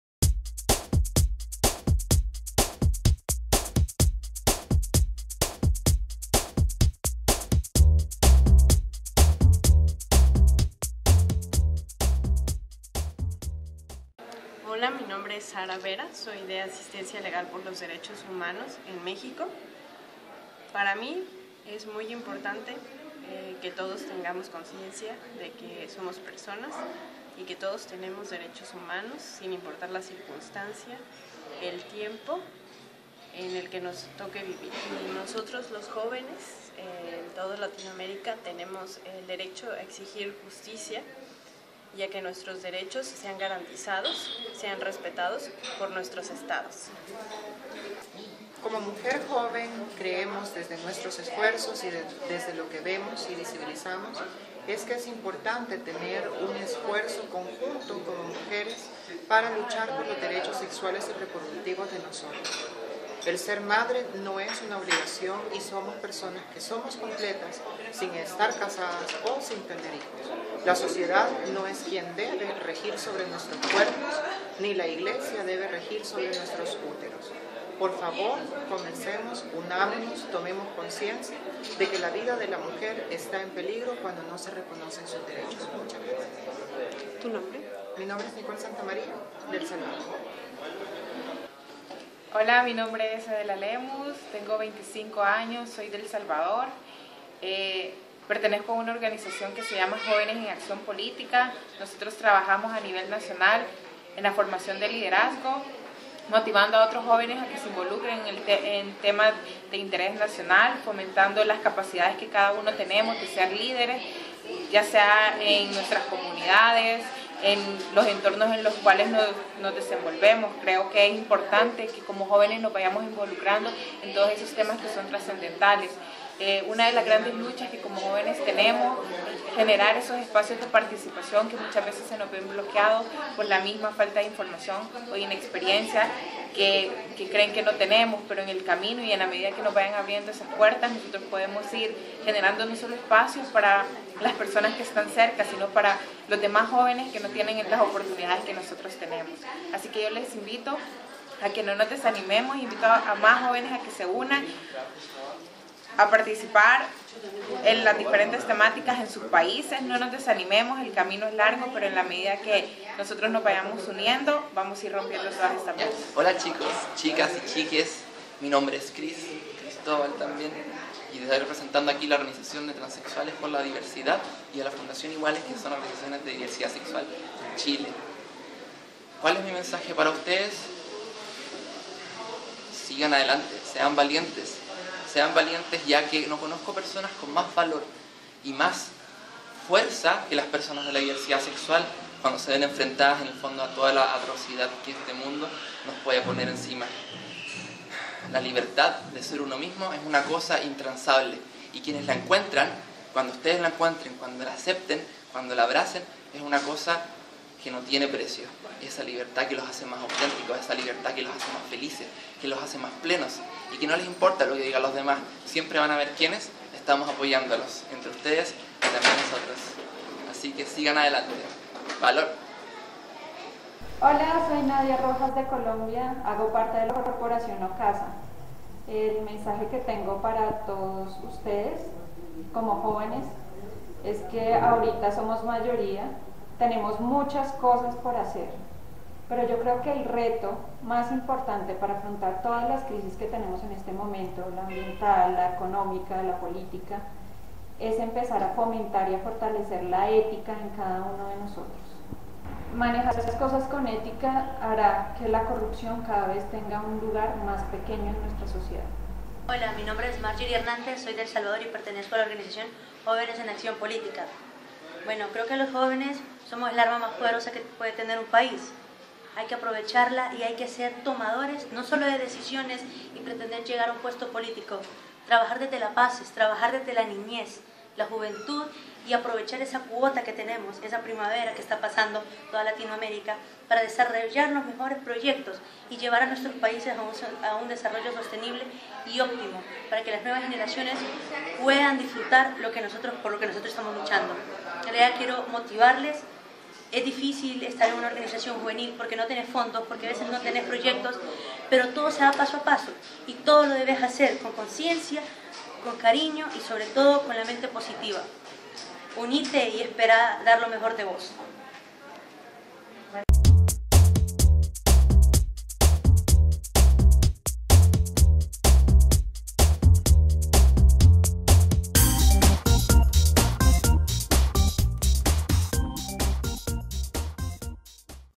De mí para ti - mensajes de jóvenes latinoamericanxs
En este video varios jóvenes de la Sociedad Civil de El Salvador, México, Chile y Colombia expresan un mensaje para sus compañerxs latinoamericanos que están en sus mismas causas.

Estxs jóvenes participaron en el TechCamp El Salvador 2014 organizado por la Comunidad de las Democracias y el Departamento de Gobierno de los Estados Unidos.